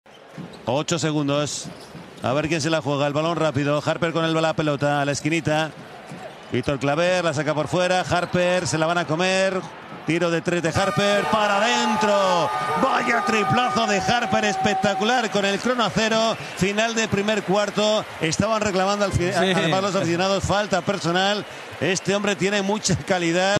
HARPER se fabrica un tiro IMPOSIBLE sin ángulo | Liga Endesa 2023-24
Jared Harper encontró un espacio imposible en la esquina. El exterior de Valencia Basket capturó el balón en desequilibrio, engañó al defensor con una finta abierta y convirtió un triple espectacular sobre la bocina del primer cuarto.